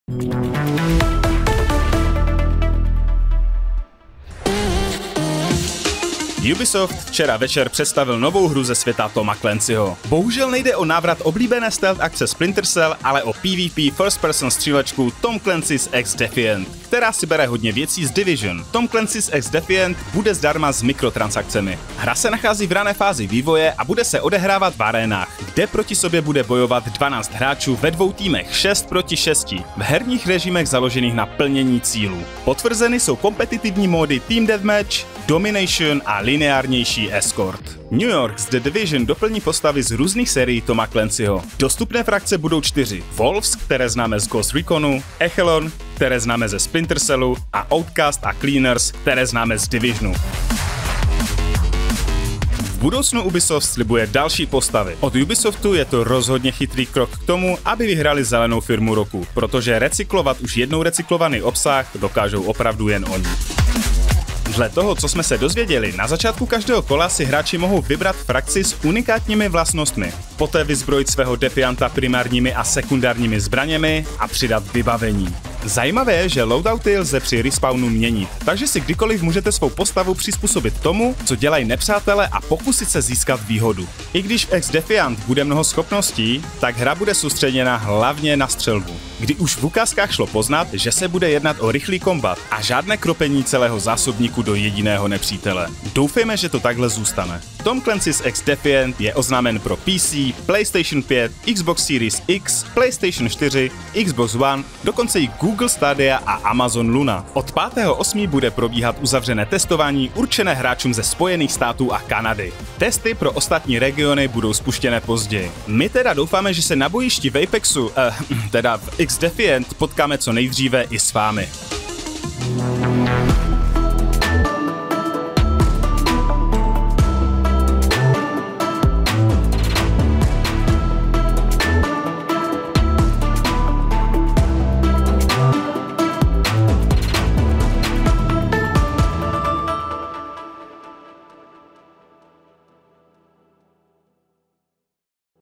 Znásilnění značky Tom Clancy's?! XDefiant je Ubisoftí APEX
Ubisoft dnes v osm večer představil novou hru ze světa Toma Clancyho. Bohužel nejde o návrat oblíbené stealth akce Splinter Cell, ale o PvP first-person střílečku Tom Clancy's XDefiant. Shcválně, jestli dovede představit něco nového.

Připravil Michal Burian a Dominik Vysloužil

Děkujeme obchodu s elektronikou CZC.cz